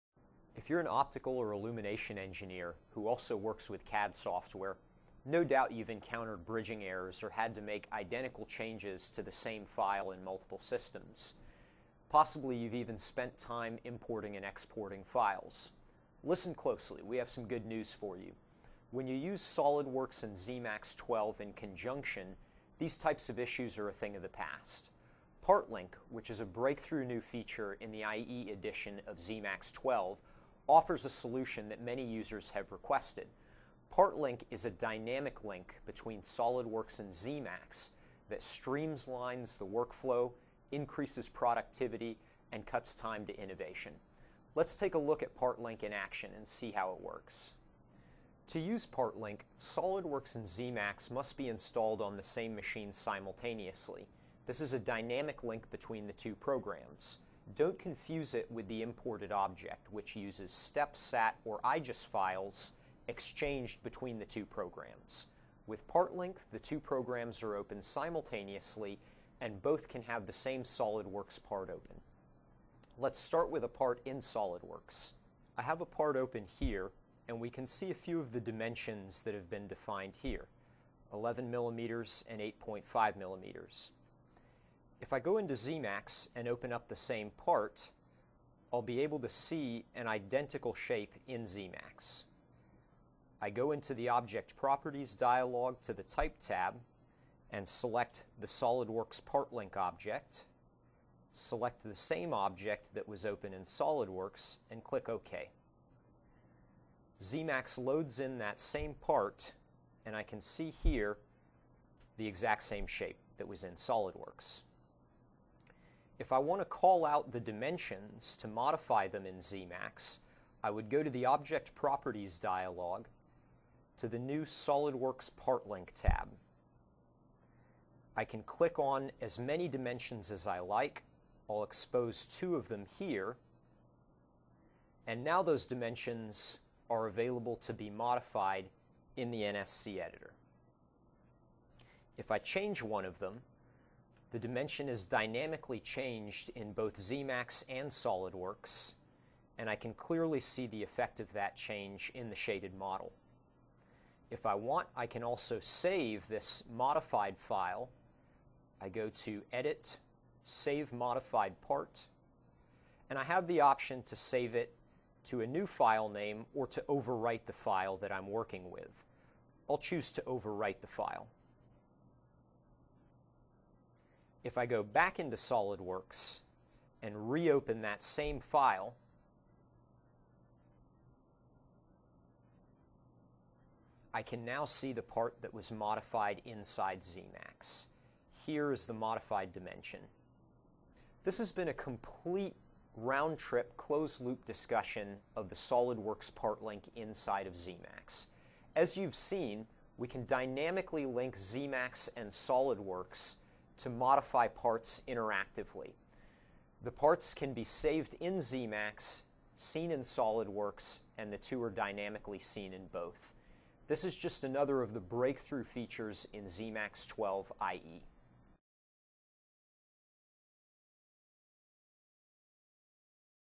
Partlink for SolidWorks - Akash Arora
If you are an Optical or illumination engineer who also works with CAD software, no doubt you've encounteredbridging errors, had to make identical changes in multiple systems, and spent time importing and exporting files, well, I have good news...
Partlink, a breakthrough feature in the IE Edition of ZEMAX 12, offers the solution users have requested.
Let's take a look at the PartLink in action...